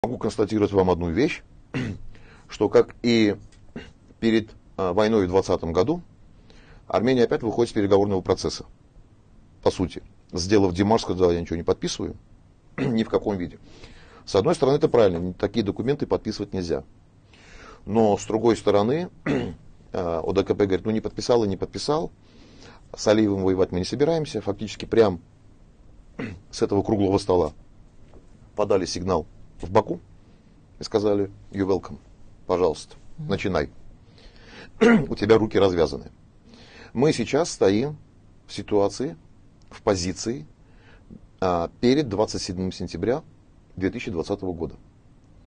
Владимир Погосян: "Армения сейчас находится в состоянии как перед войной 2020 года"
Советник бывшего начальника Генштаба Армении Владимир Погосян:

"Армения сейчас находится в состоянии как перед войной 2020 года, выходя из переговорного процесса и  сделав демарш, заявив о том, что мы ничего подписывать не будем, что, с одной стороны, правильно, поскольку такой документ подписывать нельзя. С другой стороны члены ОДКБ открыто заявили о том, что не будут воевать с Алиевым и дали ему месседж начать войну. Мы сейчас в такой же позиции, как перед 27 сентября 2020 года".